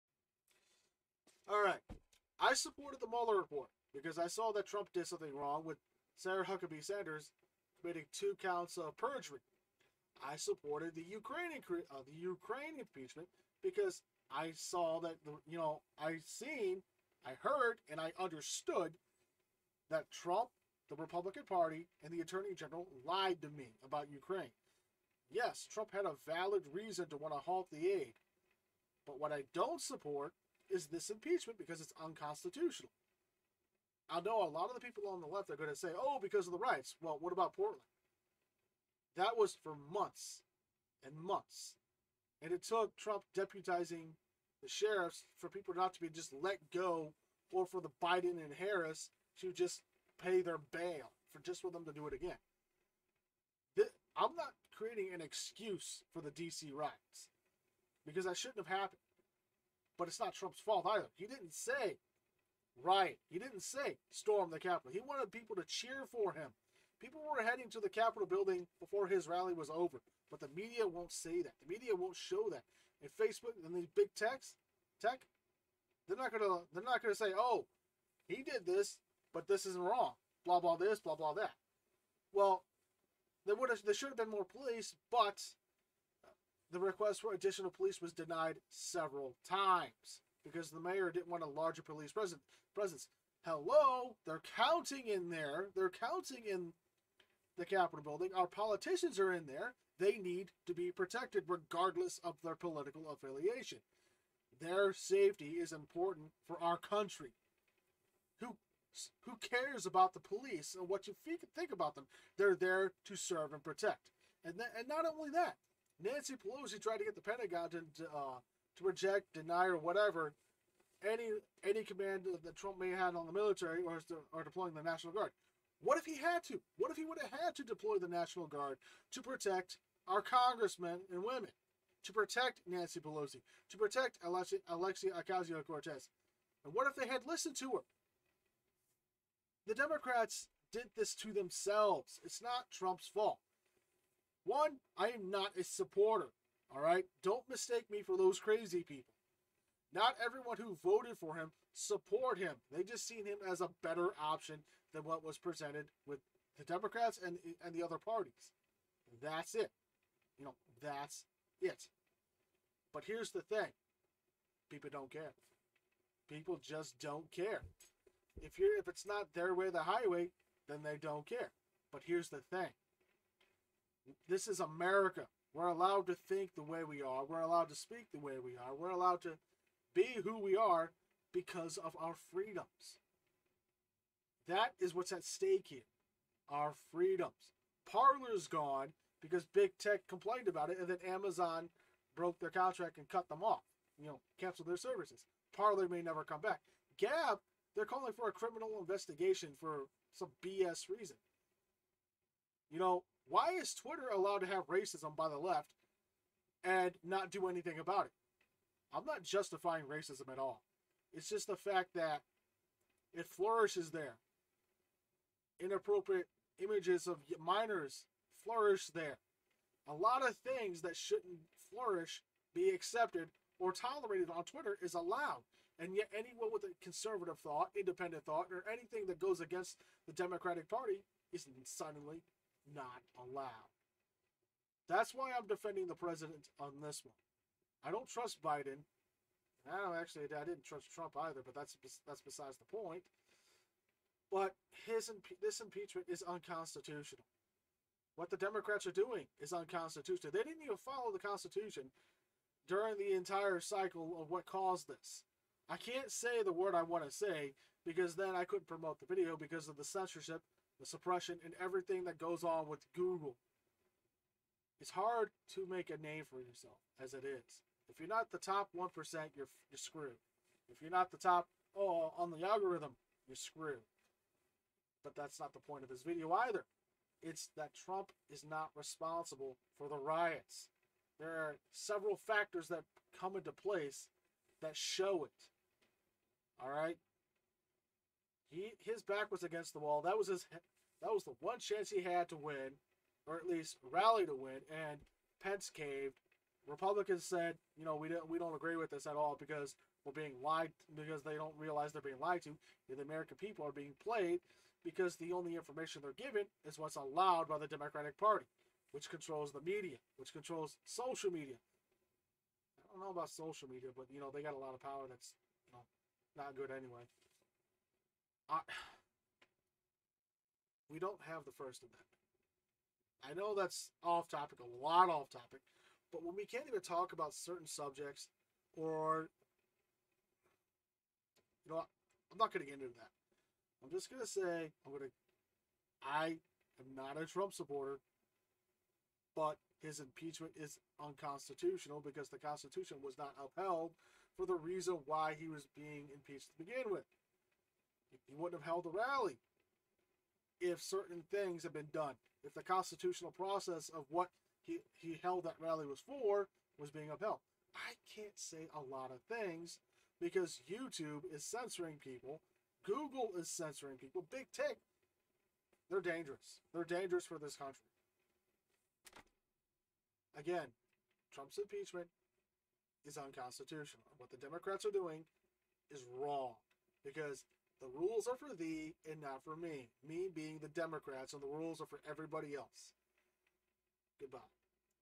Trump's second impeachment
its just wrong, he's not at fault because there where factors that prove it. factors Google will censor me for if i explain it in detail